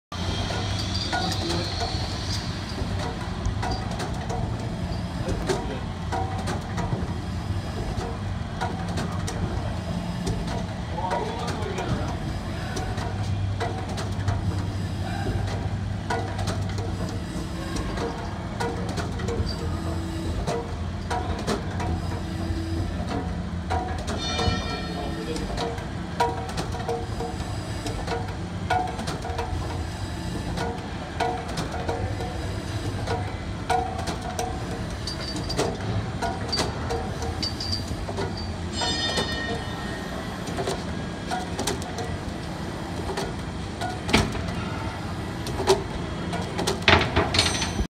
Locomotive engine inspection
Barring over a locomotive engine for inspection